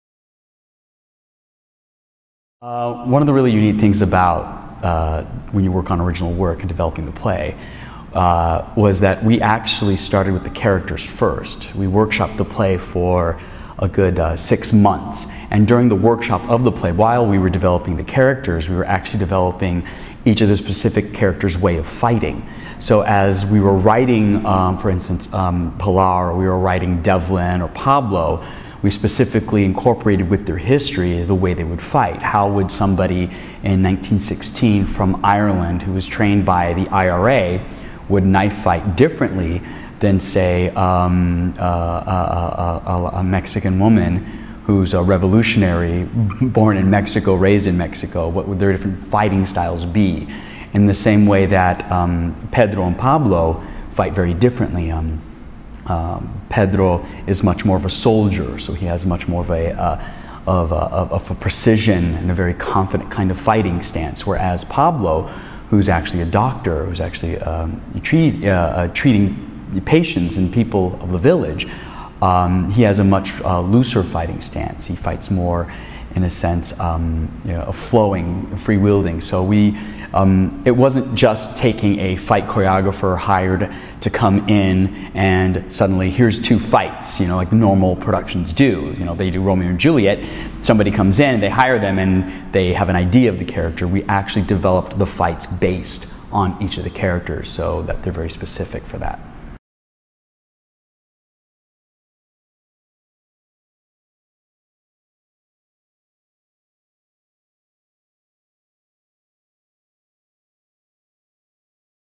Two Year Acting Program - Fight Choreography
The Maggie Flanigan Studio is a two year acting conservatory in New York City. In this video Alberto Bonilla, studio alumni and director discusses Dia De Los Muertos.